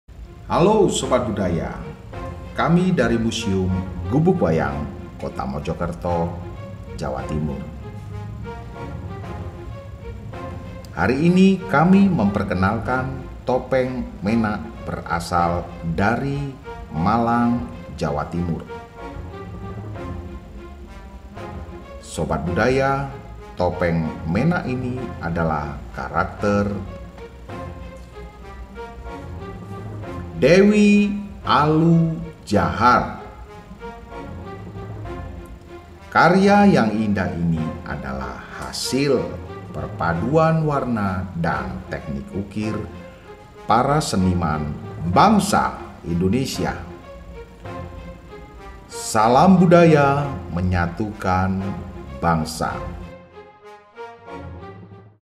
TOPENG MENAK EPS 14
TOPENGMENAK BUDAYAINDONESIA Halo Sobat budaya! bertemu lagi dengan kita tim kreatif dari Gubug Inovatif. Kali ini kita akan memperkenalkan koleksi kita yaitu TOPENG MENAK MALANGAN koleksi dari Museum Gelar Indonesia Budaya (Ganesya) yang berlokasi di Malang. Penasaran topengnya bagaimana? bisa datang ke Museum ya yang berlokasi di kompleks Hawai Waterpark Malang. Salam Budaya menyatukan Bangsa 🙏🏻